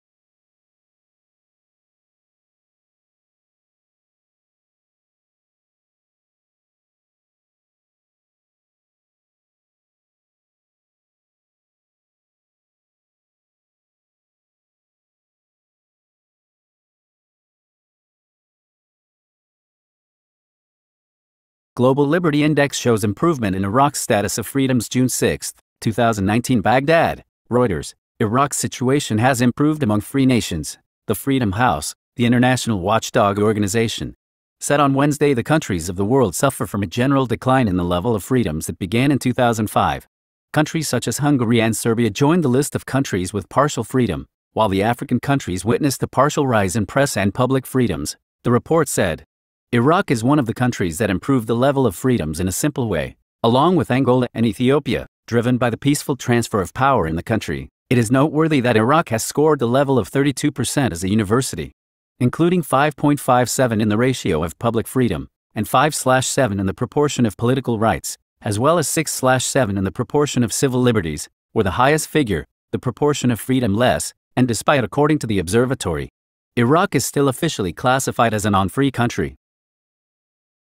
(6/6) Global Liberty Index shows improvement in Iraq’s status of freedoms
Global Liberty Index shows improvement in Iraq’s status of freedoms